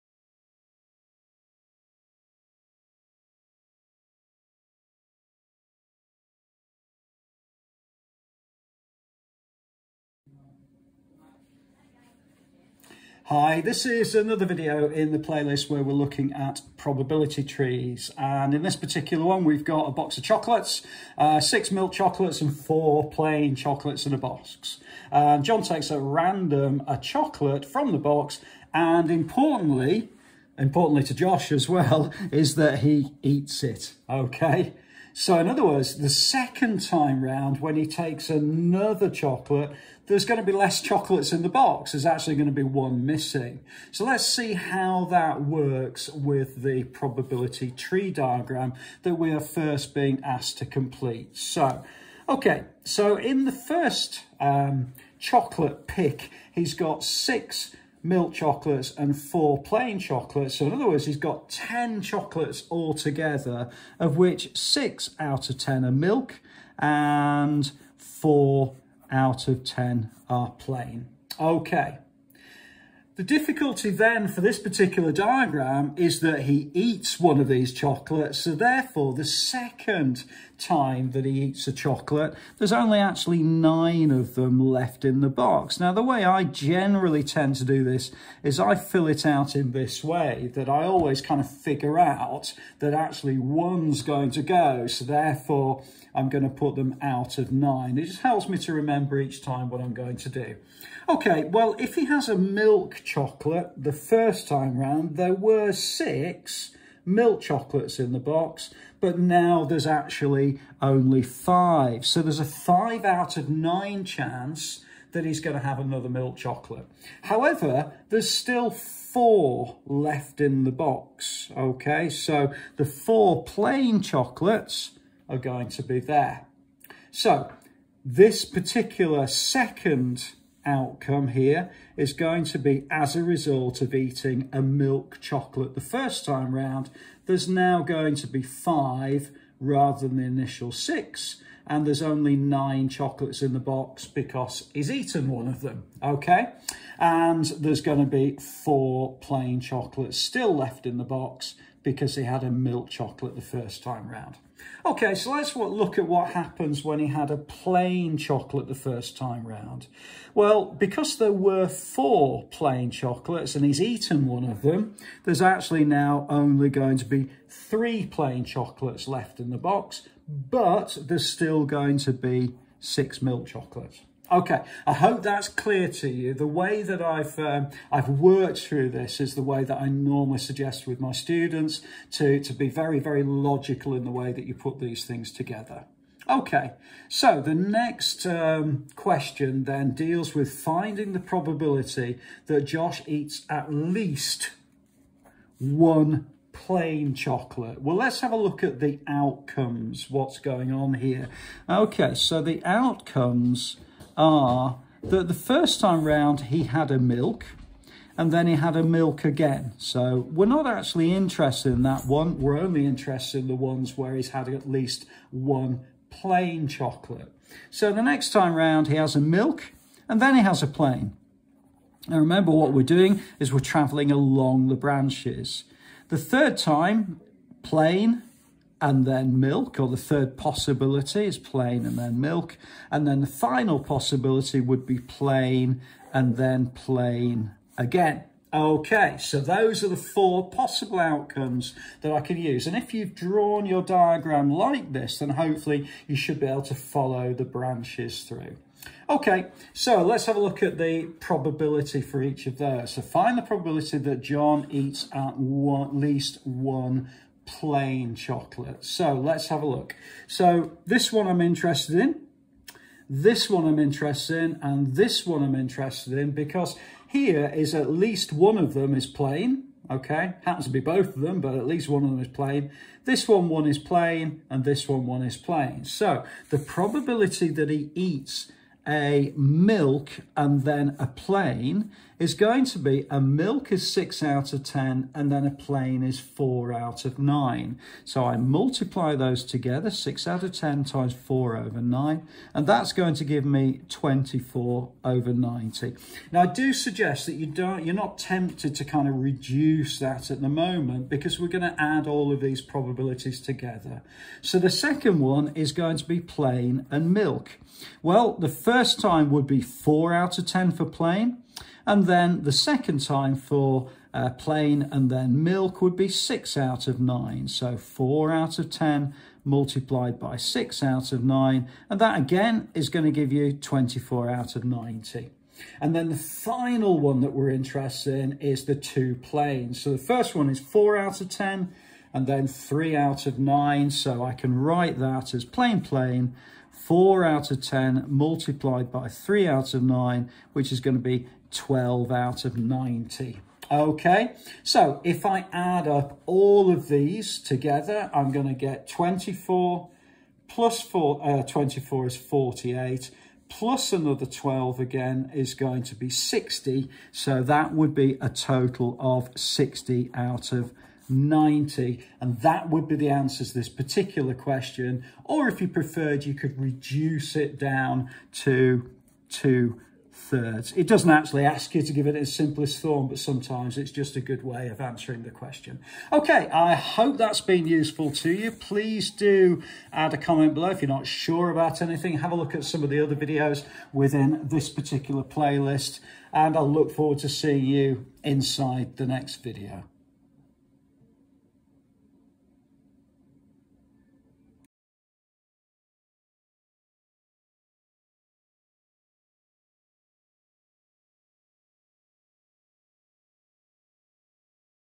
How to work with a probability tree diagram 2 - GCSE maths
@3minutemaths quick reminder and GCSE maths walkthrough videos.

This video is all about how to work with a probability tree diagram and forms part of the playlist of the same name. The question ios aimed at around grade 5 GCSE maths and should be fairly straightforward if you have practiced similar questions.

There are 6 milk chocolates and 4 plain chocolates in a box.
Josh takes a random chocolate from the box and eats it.
He then takes another chocolate and eats that as well.

a) Complete the probability tree diagram.
b) Find the probability that Josh eats at least one plain chocolate.